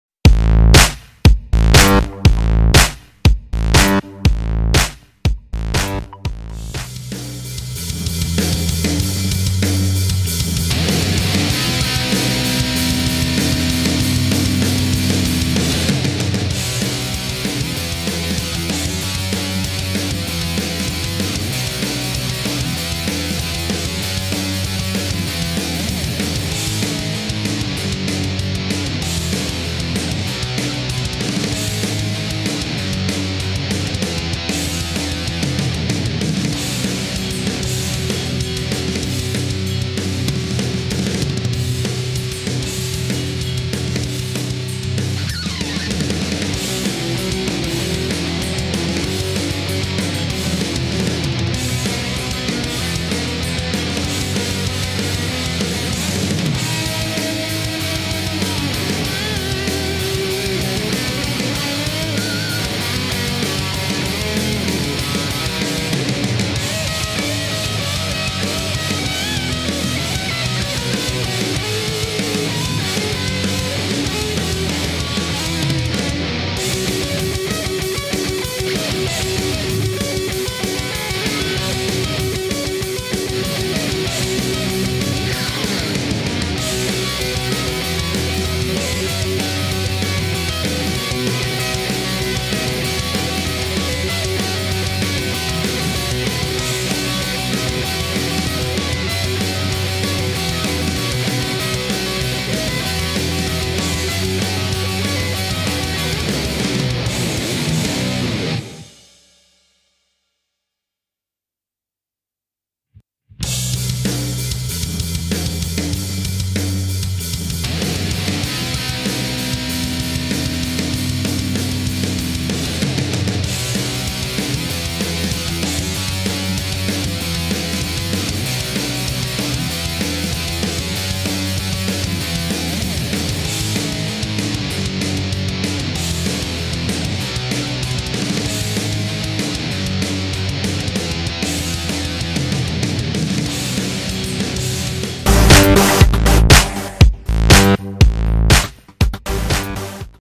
Conversano - Teramo 28-20 | La sintesi
Le più belle azioni della gara della 7^ giornata di Serie A Femminile, vinta 28-20 dal Conversano contro la Nuova Teramo.